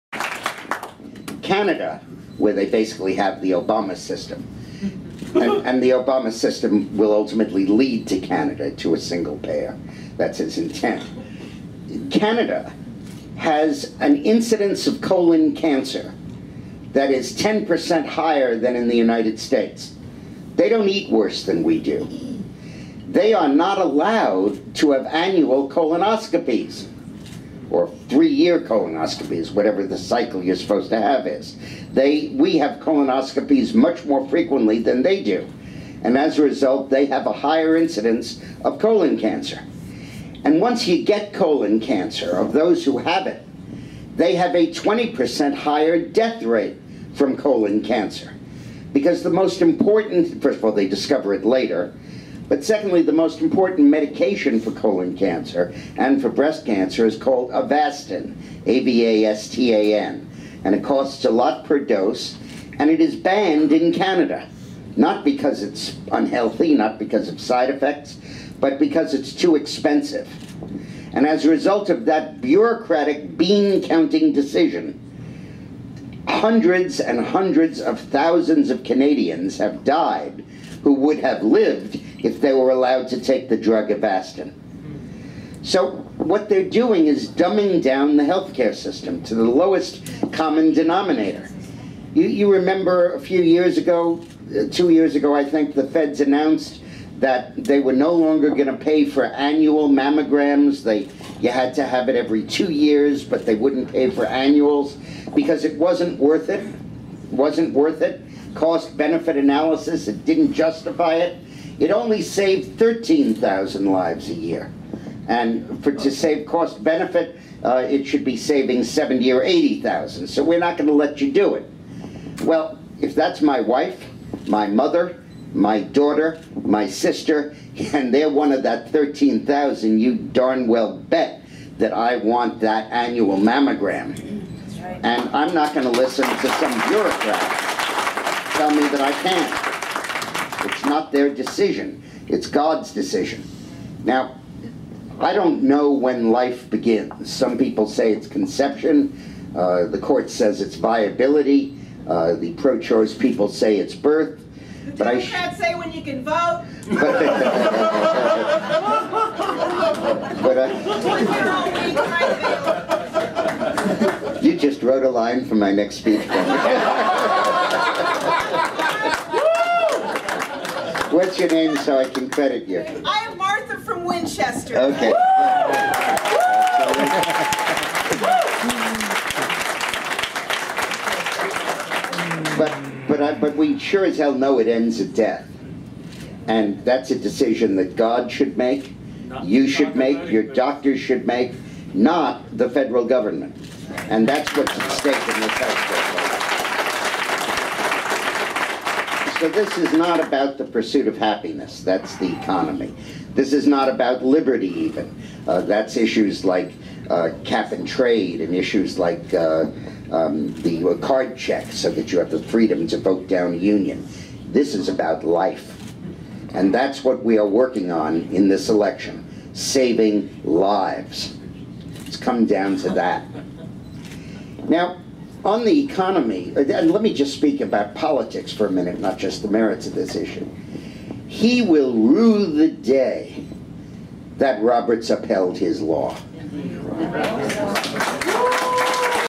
Dick Morris AFP Sterling, VA 30Jun2012 2 of 7
Morris reacts to the SCOTUS ObamaCare decision, and the state of the election campaign in general.